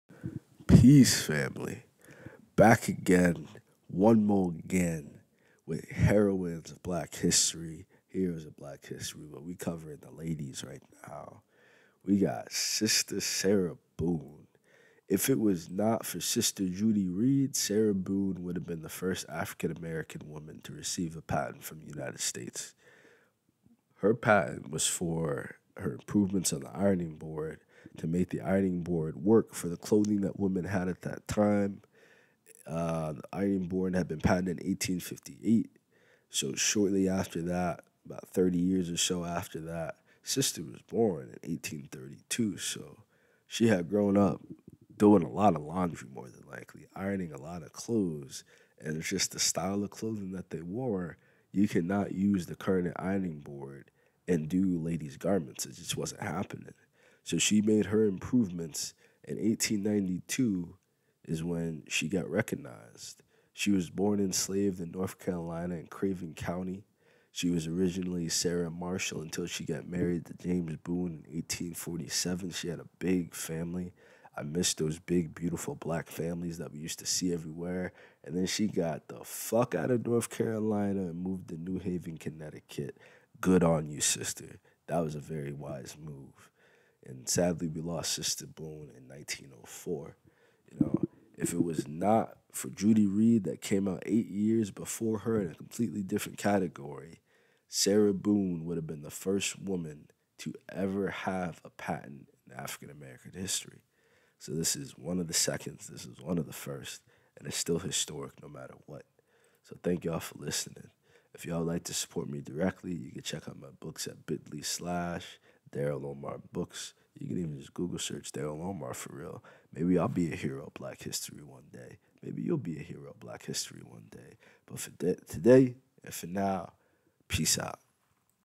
Sarah Boone- Heroes of Black History 2024
A former slave-turned-dressmaker invented an innovation to the ironing board. Her invention revolutionized how women were able to iron their garments and impacted the new design of the ironing board. Sarah got out of the south and moved to the north.